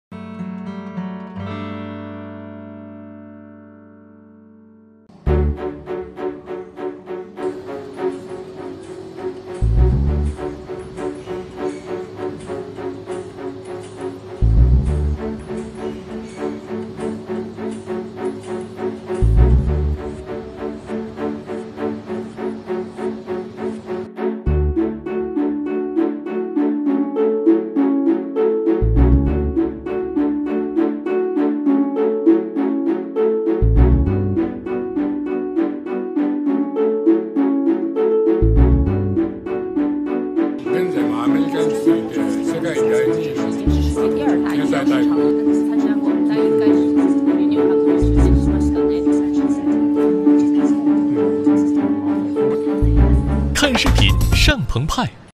The world’s thinnest and shortest needle from Japan is on display at CIIE
The world’s thinnest and shortest needle from Japan’s Terumo is on display at CIIE in Shanghai. Only 3mm in length and 0.18mm in diameter, it is expected to reduce the pain of diabetes patients when injecting insulin.